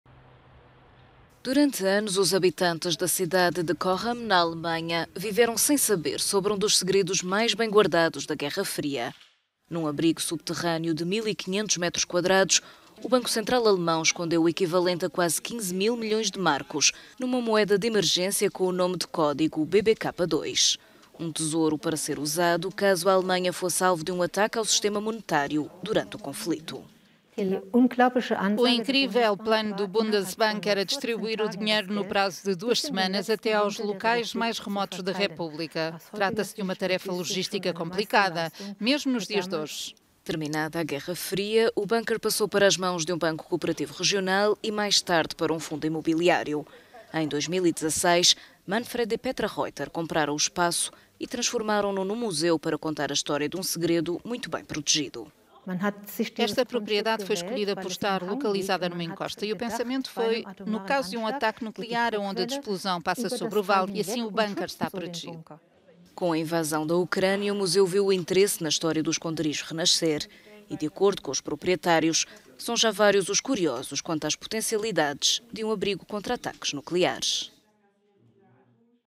Alemanha revela segredo da Guerra Fria
Num abrigo subterrâneo, o Banco Central Alemão escondeu durante anos um tesouro: notas num montante equivalente a 15 mil milhões de marcos